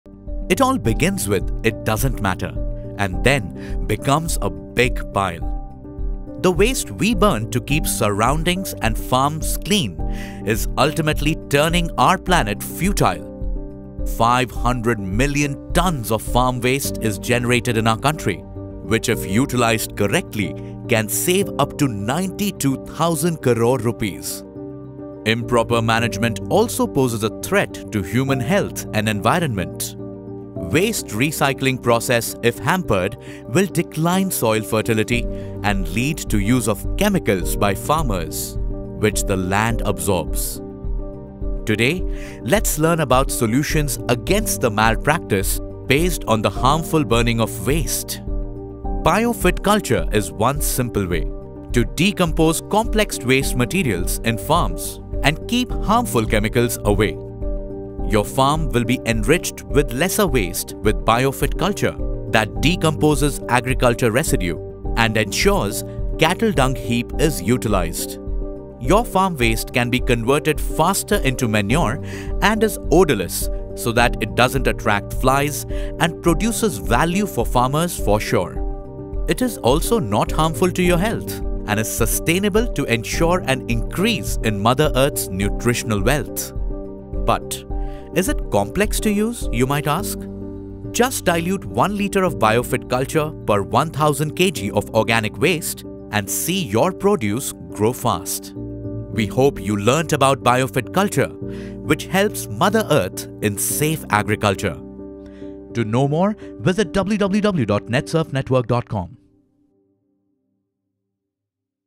Biofit Culture - English
Presenting Biofit Culture, an environmentally sustainable solution for waste decomposition! Biofit Culture helps in the rapid, complete, and total decomposition of organic waste into ready-to-use, nutrient-rich odorless soil manure. Biofit Culture can be used for the decomposition of agriculture residue, cattle dung heap, and organic kitchen and housing society waste without the use of potentially harmful chemicals.